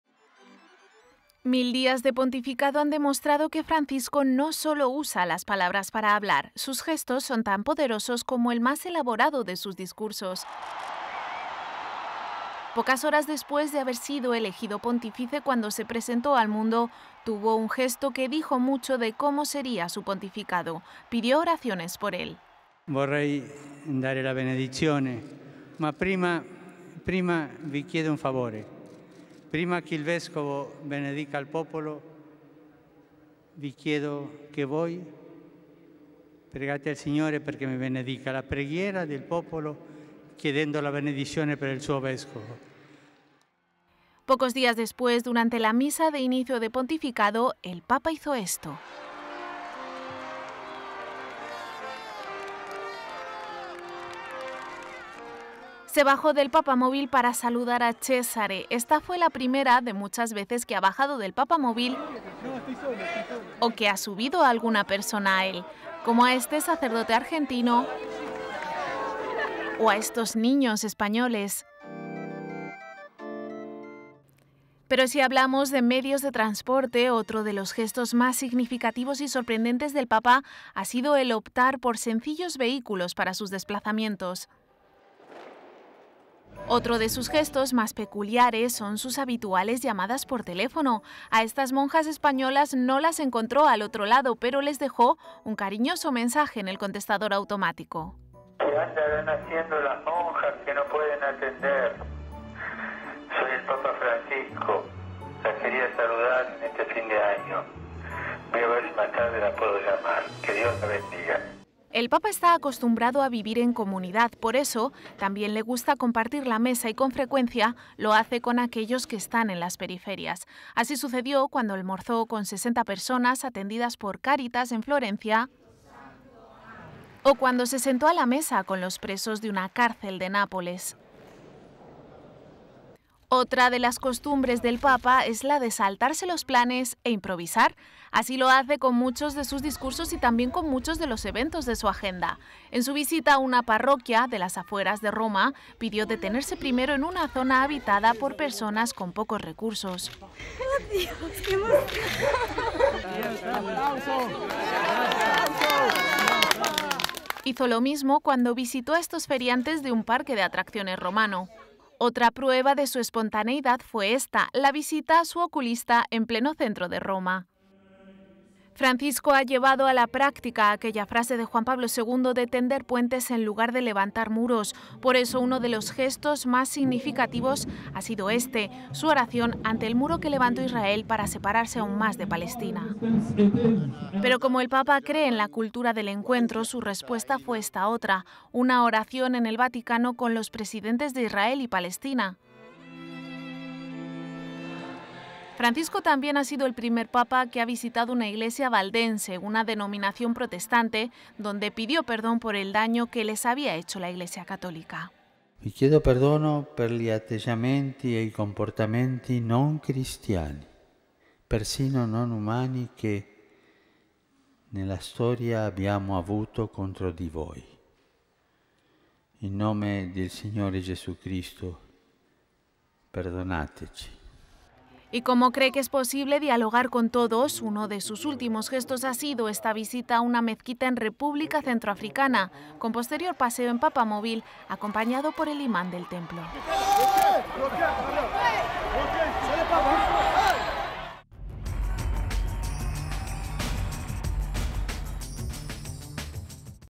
Los 7 gestos más impactantes de los 1.000 días de Francisco como Papa
5 de diciembre, 2015. Los 7 gestos más impactantes de los 1.000 días de Francisco como Papa